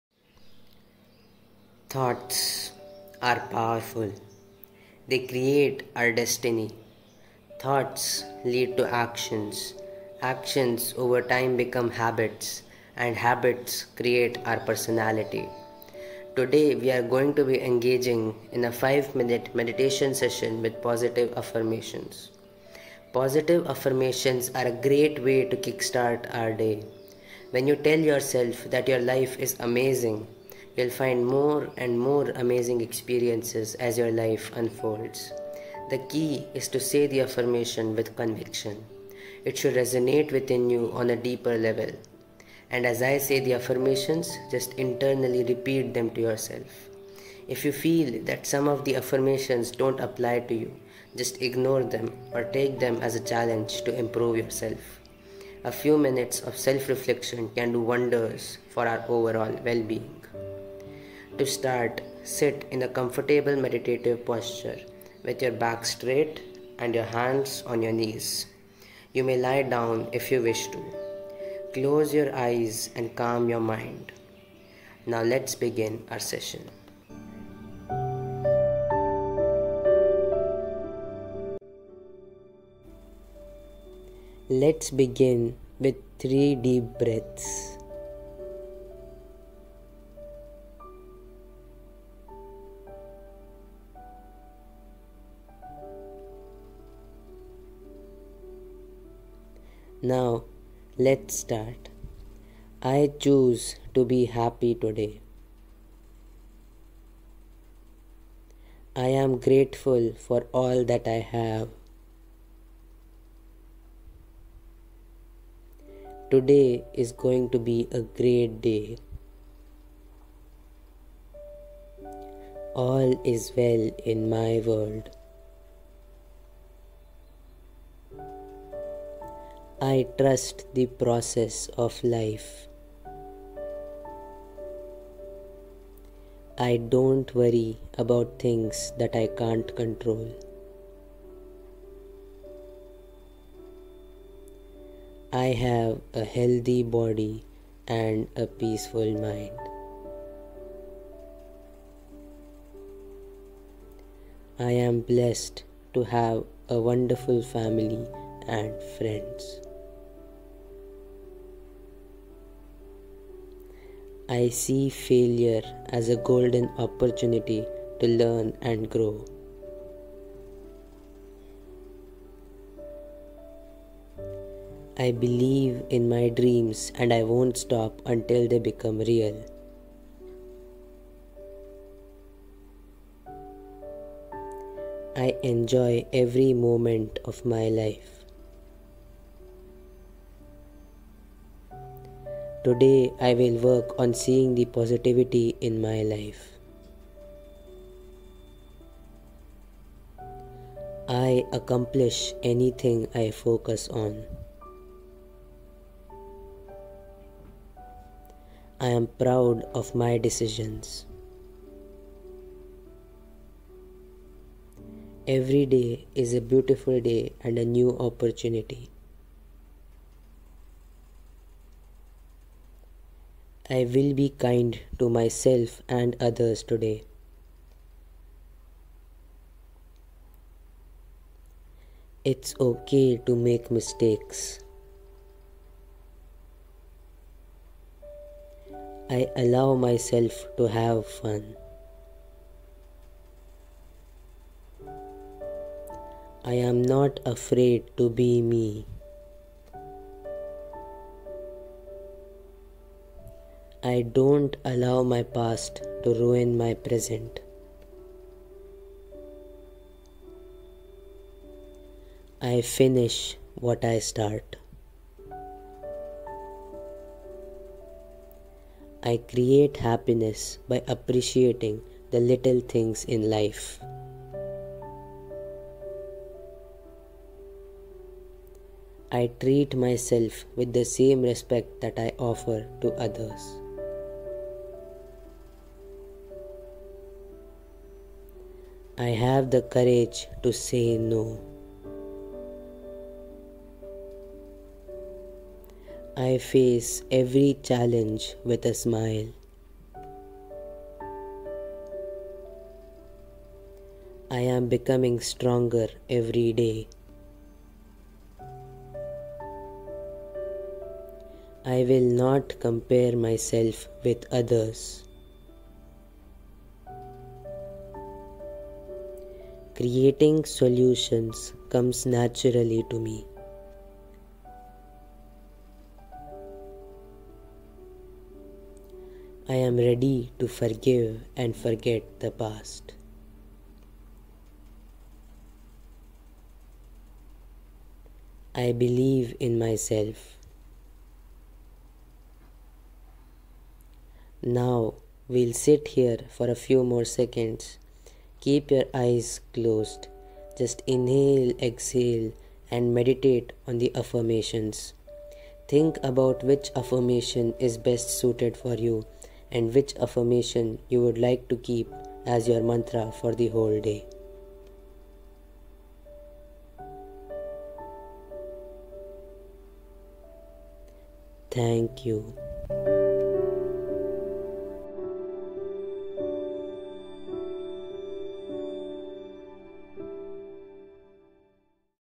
A Dose of Positivity || 5 min Meditation Session with Positive Affirmations
A 5 minute Meditation Session with Positive Affirmations to help you harness the power of Positive Thinking 😊

Our thoughts create our destiny. Thoughts are powerful. Thoughts lead to actions. Actions over time become habits. And habits create our personality.
Positive Affirmations are a great way to kick-start your day. When you tell yourself that your life is amazing, you’ll find more and more amazing experiences as your life unfolds. Positive affirmations are very powerful because they release you from negativity, fear, worry, and anxiety. When these affirmations are repeated over and over again, they begin to take charge of your thoughts, slowly changing your pattern of thinking and ultimately changing your life. Self-affirmation also helps to mitigate the effects of stress. The key is to say the affirmation with conviction. It should resonate within you on a deeper level. A few minutes of self reflection can do wonders for your overall WellBeing.

To begin the session - Sit in a comfortable meditative posture with your back straight and your hands on your knees. You may lie down   if you wish to. Close your eyes and calm your mind.
During the session - As I say the affirmations just internally repeat them to yourself. If you feel that some of the affirmations don't  apply to you just ignore them or take them as a challenge to improve yourself.
After the session - Sit in the meditative posture for a few more seconds. Keep your eyes closed. Just Inhale & Exhale and meditate on  the affirmations. Think about which affirmation is best suited for you and which affirmation you would like to keep as your mantra for the whole day. Then slowly open your eyes and have a great day 😁

Positive Affirmations -
 • I choose to be happy today
 • I am grateful for all that I have
 • Today is going to be a great day
 • All is Well in my world
 • I trust the process of life
 • I don't worry about things that I can't control
 • I have a healthy body and a peaceful mind
 • I am blessed to have wonderful family and friends
 • I see failure as a golden opportunity to learn and grow
 • I believe in my dreams and I won't stop until they become real
 • I enjoy every moment of my life
 • Today I will work on seeing the positivity in my life
 • I accomplish anything I focus on
 • I'm proud of my decisions
 • Everyday is a beautiful day and a new opportunity
 • I will be kind to myself and others today
 • It's okay to make mistakes
 • I allow myself to have fun
 • I am not afraid to be me
 • I don't allow my past to ruin my present
 • I finish what I start
 • I create happiness by appreciating the little things in life
 • I treat myself with the same respect that I offer to others
 • I have the courage to say no
 • I face every challenge with a smile
 • I am becoming stronger every day
 • I will not compare myself with others
 • Creating solutions comes naturally to me
 • I am ready to forgive and forget the past

Regards,
WellBeing